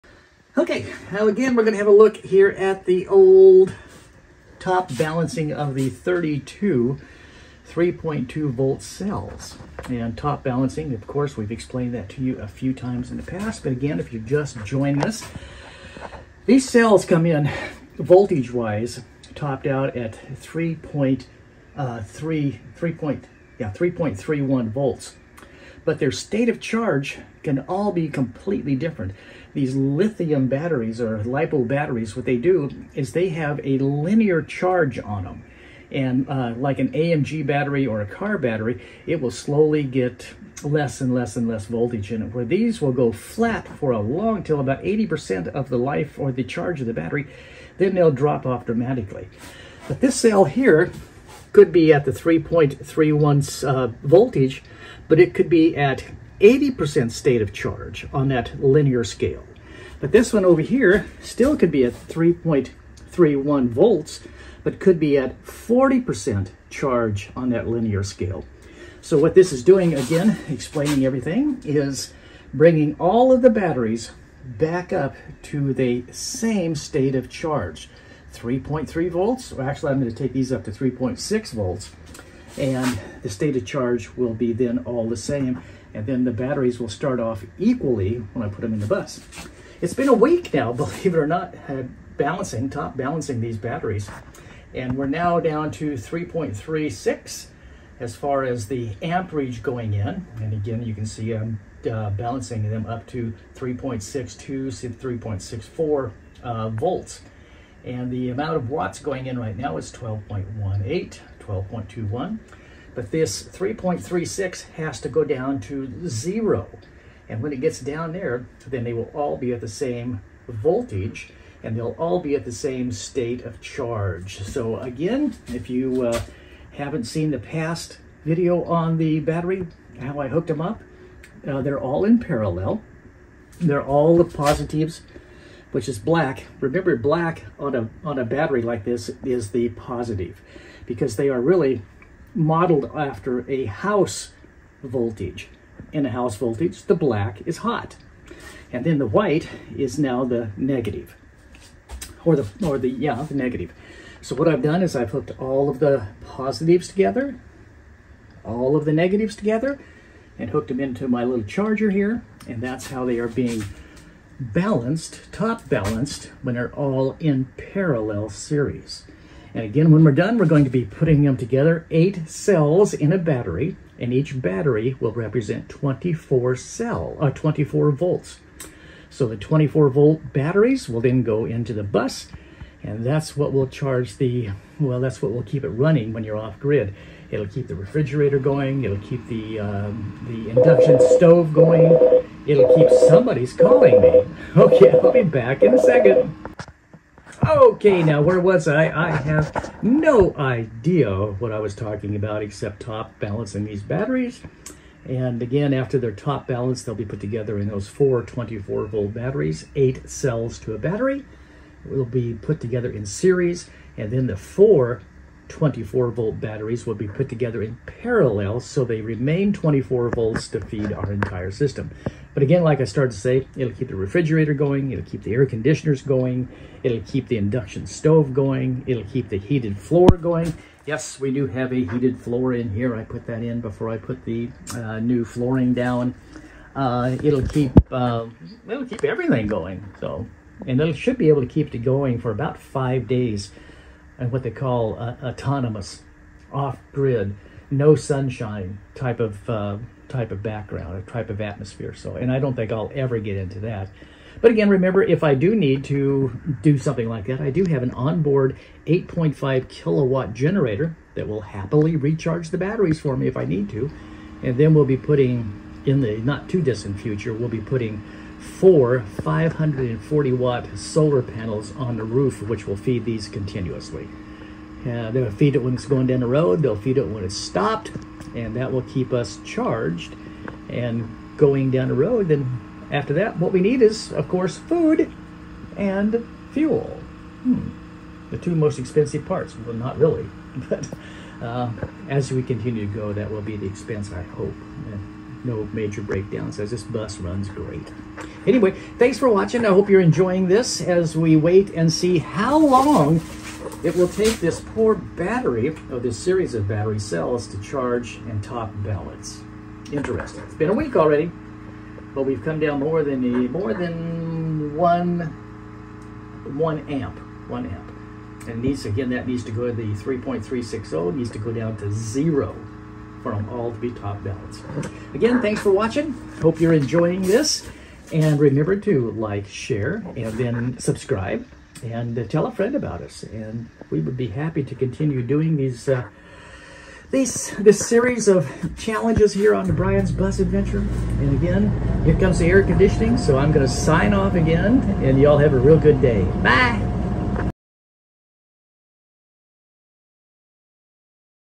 Lipo4 batteries #3.watching the top balance.
As we continue watching the lipo4 cells charge and top balance.  Over a week now,  How long? anyones guess. Worse than watching paint dry. lol